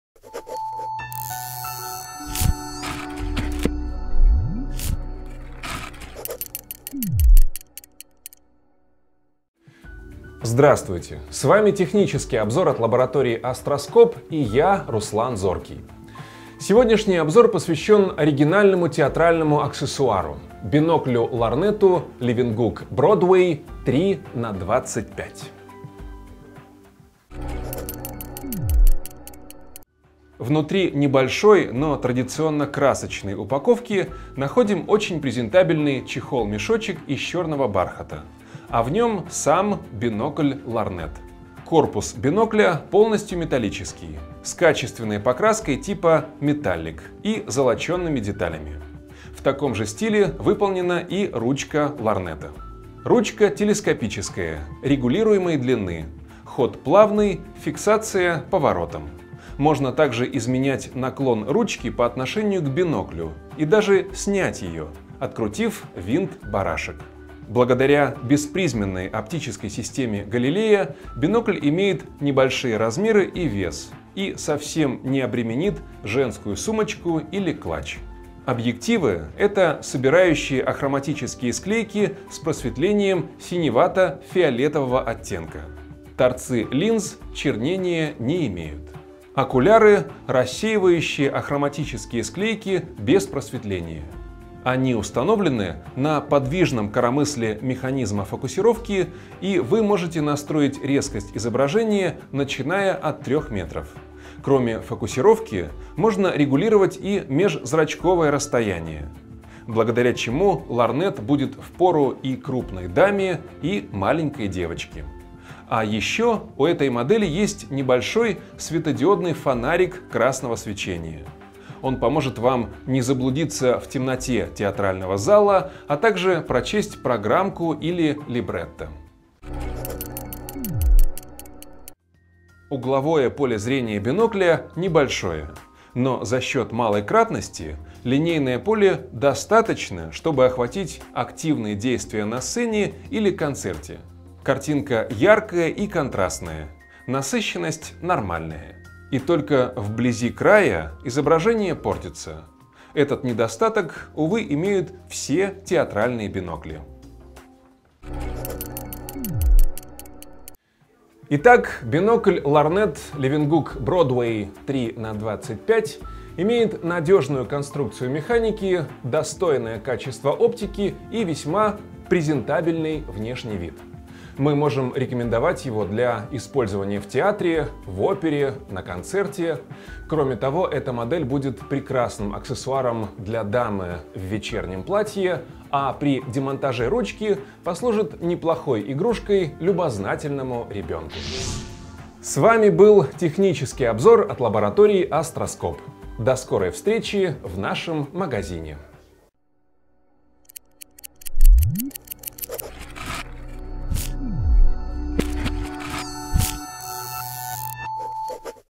Обзор бинокля Levenhuk Broadway 3х25
Levenhuk Broadway 325N – удобный бинокль-лорнет в строгом красном корпусе с яркой отделкой под золото наверняка понравиться не только дамам, но и их кавалерам. Встроенная подсветка очень удобна: с одной стороны она поможет вам сориентироваться в темном зале, а с другой – не потревожит других посетителей. Ручка лорнета удобно лежит в руках, а его малый вес не отяготит даже самые слабые руки.